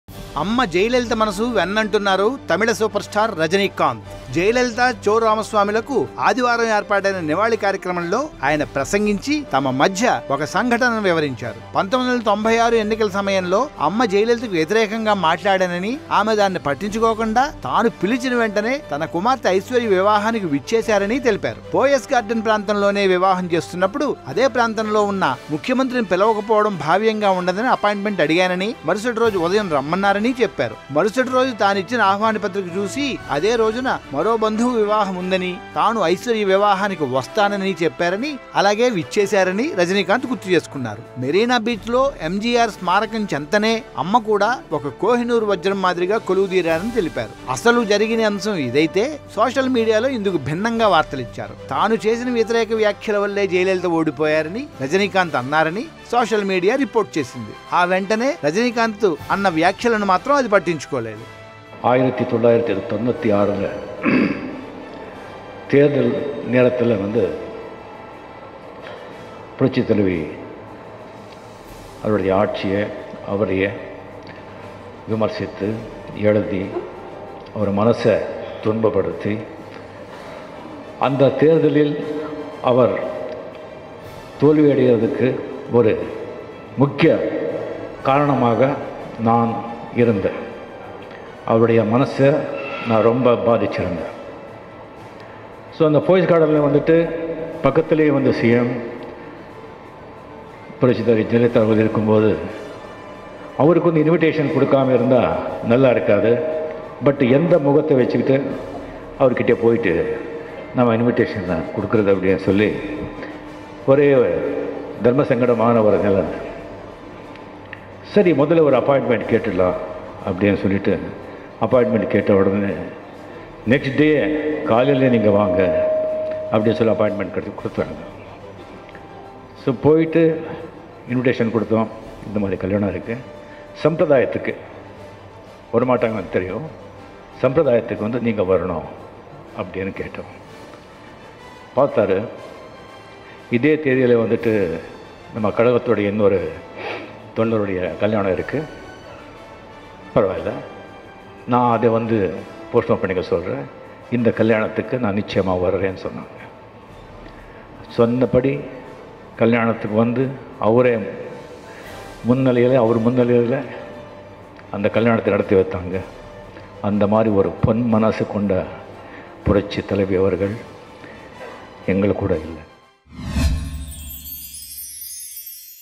Super Star Rajinikanth says 'Jayalalitha is Diamond' | in SIAA Mourning
Tamil 'Superstar' Rajinikanth on Sunday paid rich tributes to late Chief Minister Jayalalithaa, describing her as a 'kohinoor diamond' who had made her way up through difficulties in a male-dominated society.